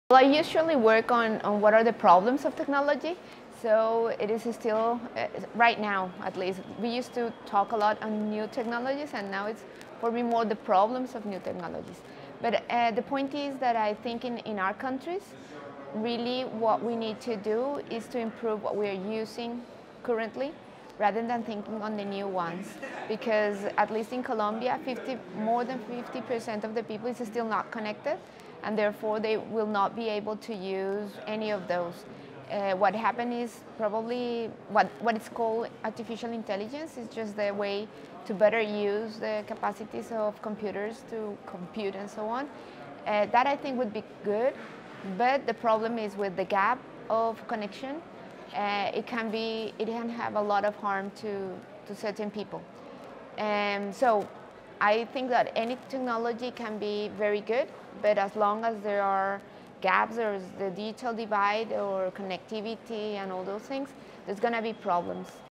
Global IGF 2018: Carolina Botero Cabrera on Best New Digital Development
Carolina Botero Cabrera, executive director, Fundación Karisma, Colombia, shares their answer to the question, “What new digital technologies will create the best opportunities for a better world all over the next 10 years?” Recorded at the Global Internet Governance Forum 2018 in Paris, France, by the Imagining the Internet Center, an initiative of Elon University, North Carolina, USA.